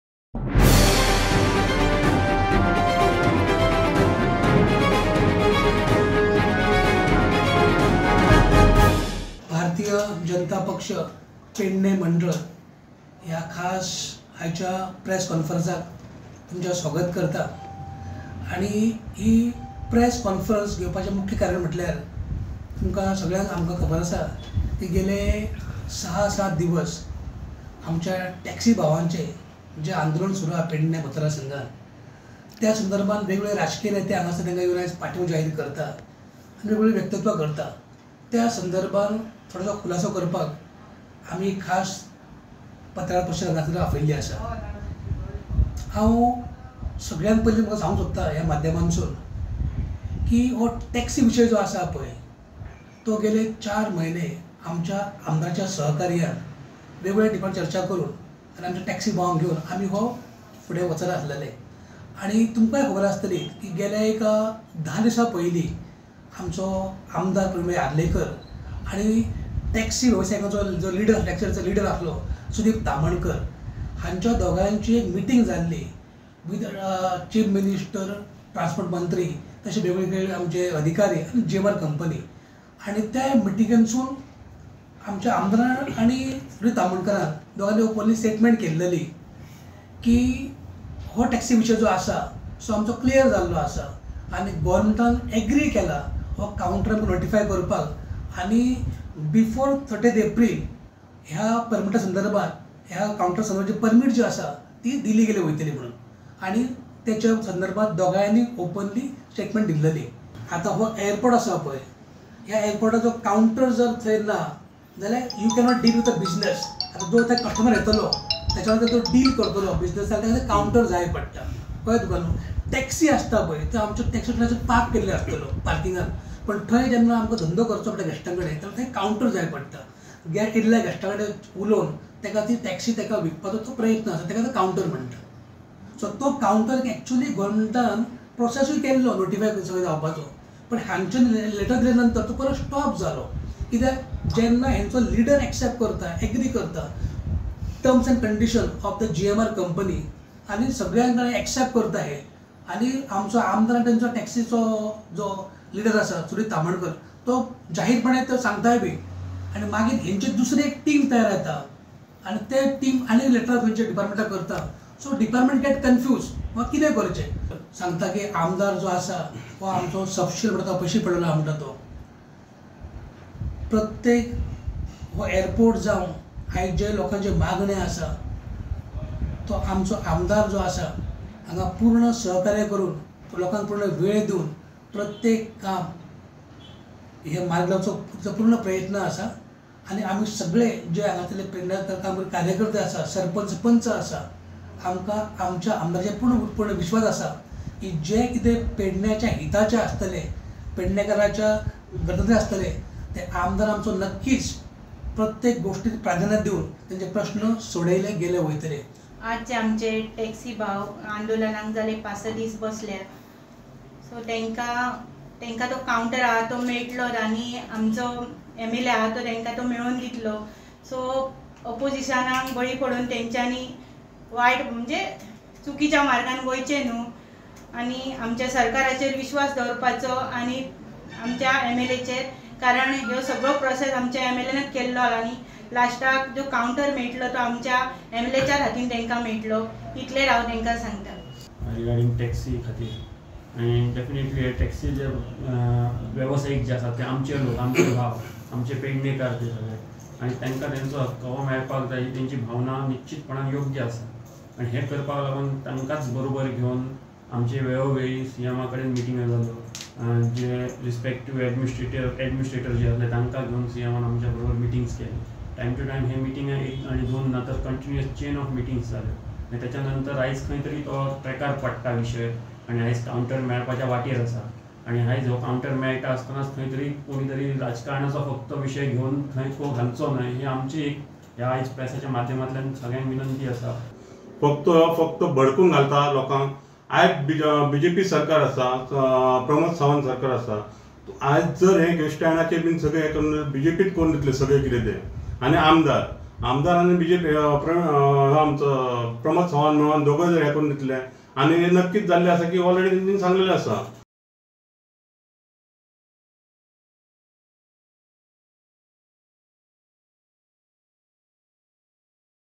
MOPA TAXI ISSUE POLITICAL
Pernem BJP Mandal alleges some political leaders are instigating taxi operators